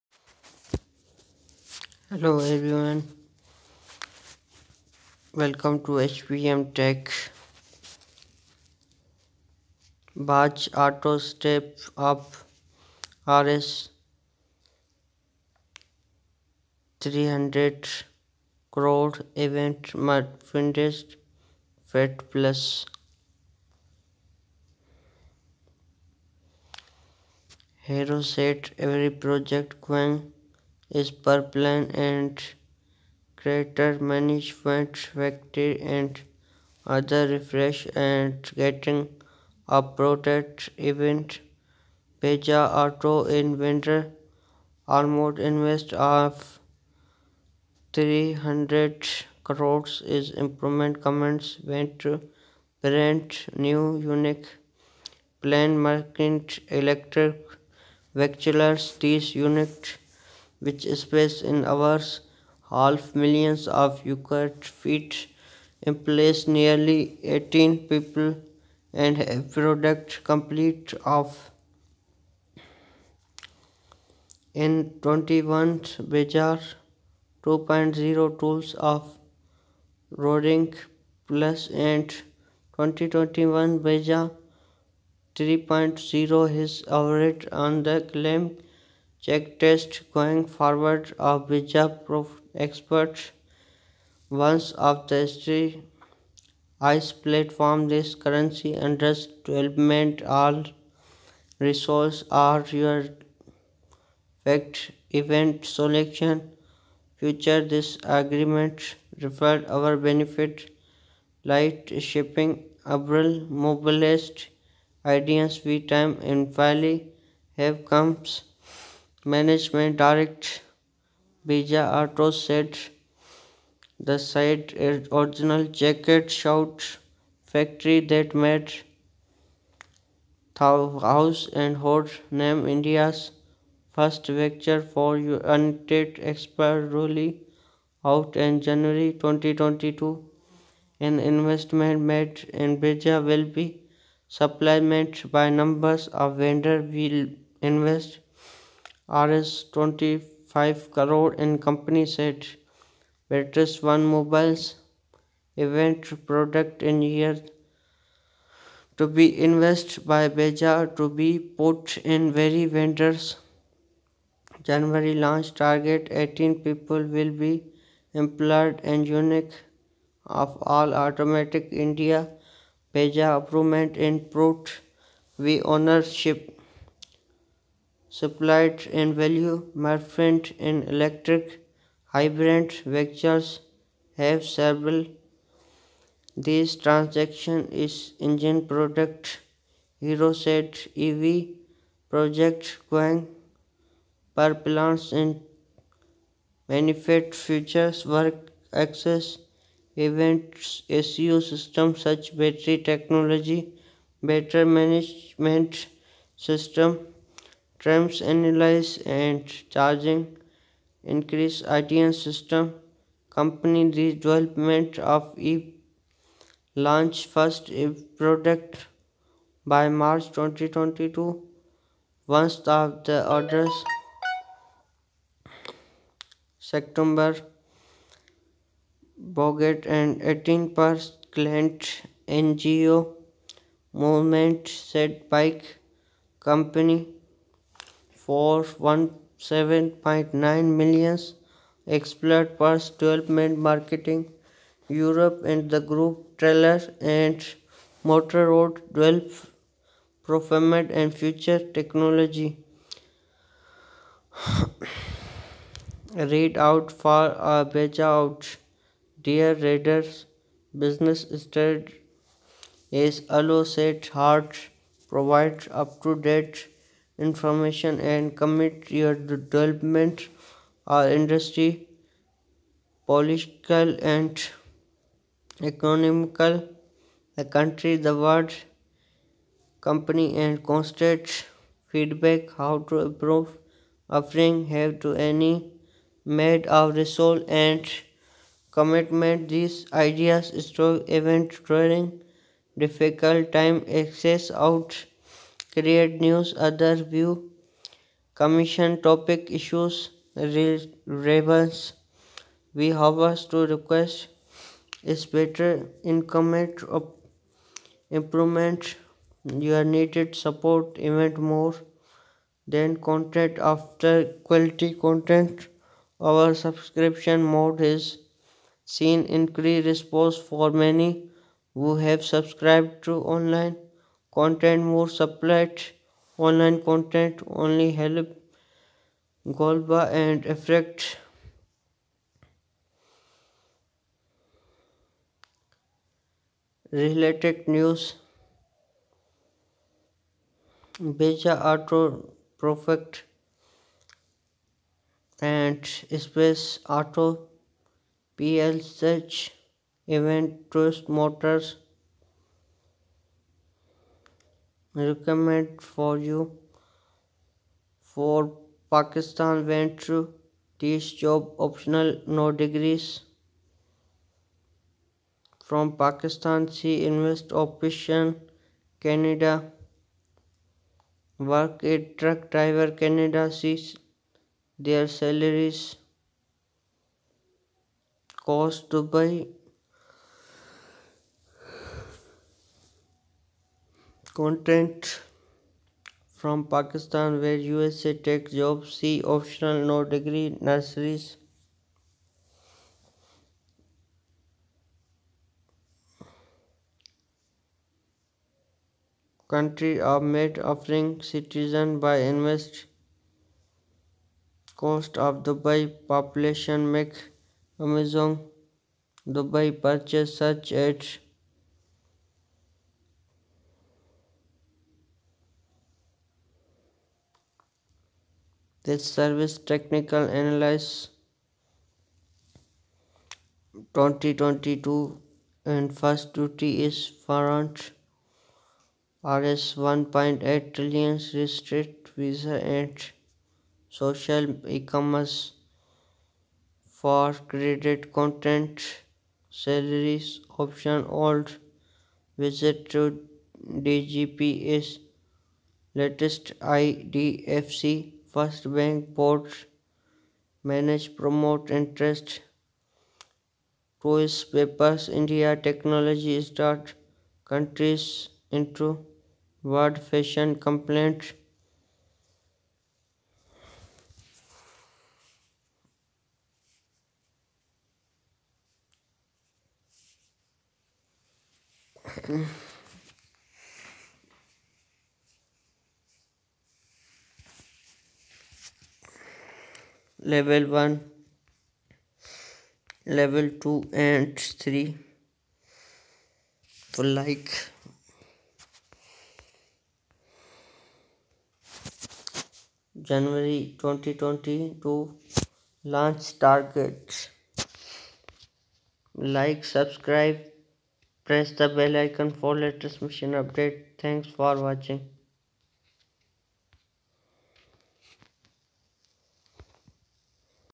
Auto sets up Rs 300 crore EV manufacturing || hpm tech
Auto sets up Rs 300 crore EV manufacturing

EVs
Bajaj Auto on Wednesday announced an investment of Rs 300 crore as it commenced work at its brand-new unit at Akurdi, Pune, for manufacturing electric vehicles (EVs). The unit, which is spread over an area of half a million square feet, will employ nearly 800 people and have a production capacity of 500,000 EVs a year.